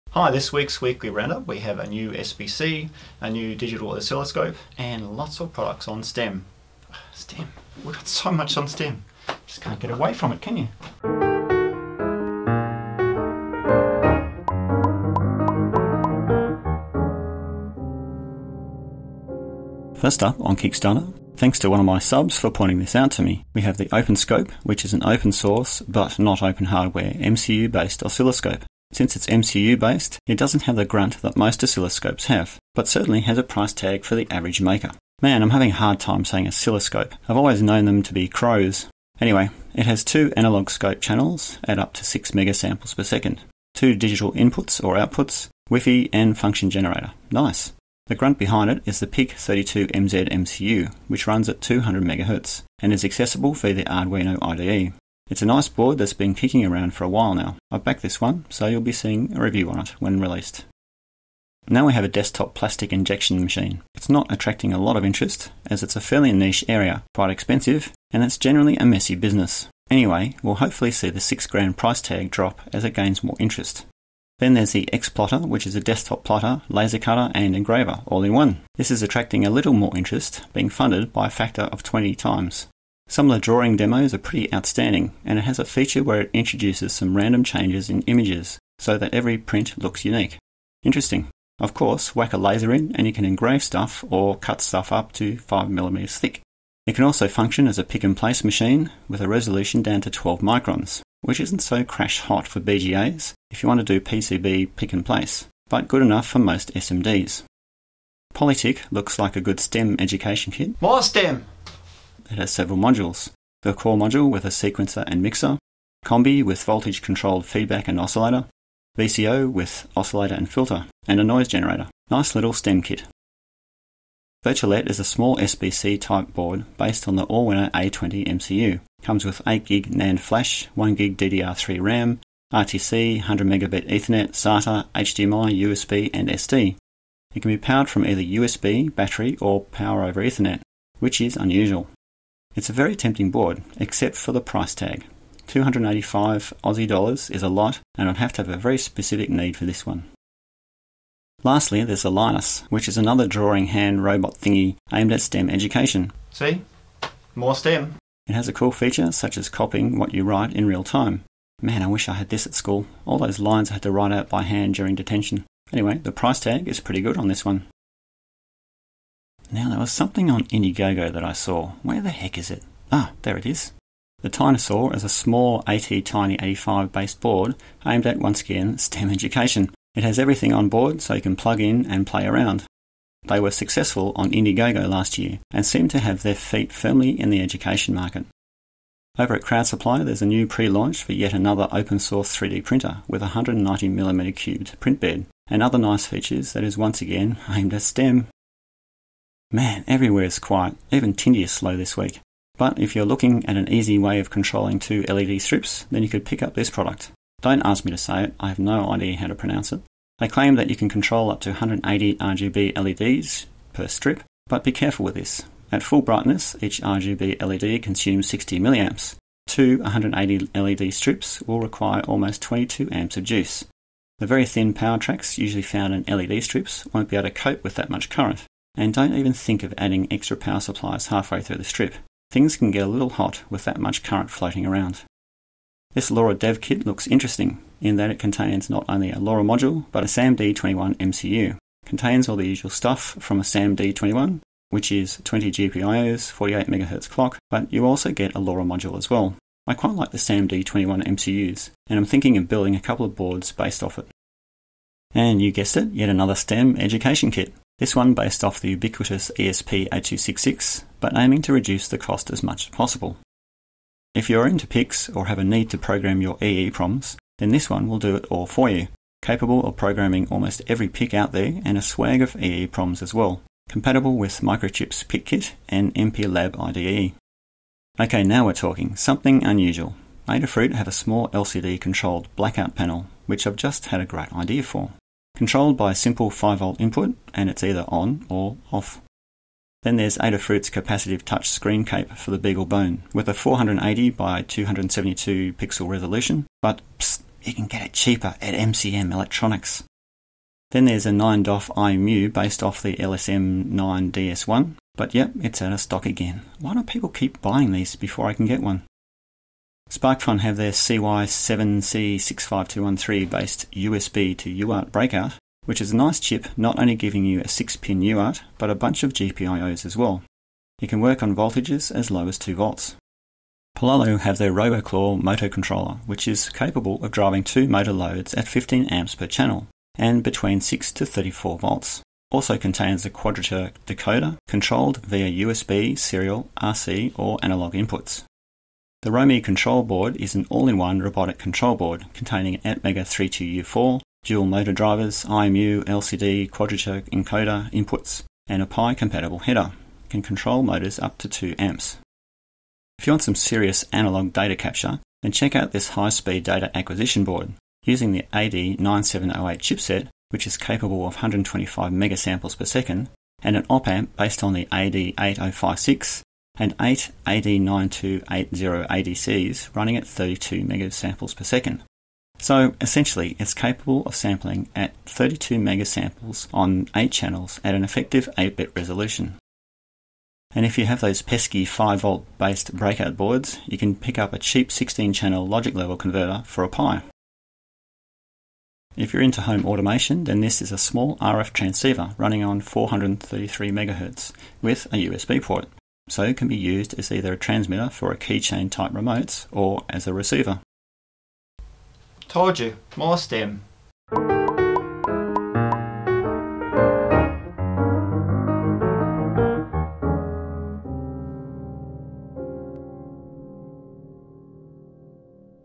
#099 Weekly Roundup #22 - New Maker Products
This week's roundup there's loads of STEM products, but also a new SBC and digital oscilloscope.

See below for links!

# Kickstarter
OpenScope: Instrumentation for Everyone

# IndieGoGo
Tinusaur

# CrowdSupply
Go-Make Junior

# Tindie
NusbioMCUpixels

# AdaFruit, Seeed, SparkFun, DFRobot, DigiKey
Liquid Crystal Light Valve

# The Cheap Side
High Speed AD/DA Module Data Acquisition Signal Source FPGA Development Board
16-Channel Level Conversion Module For Raspberry Pi
433Mhz Decoding Transceiver USB Transfer Frequency Module For Smart Home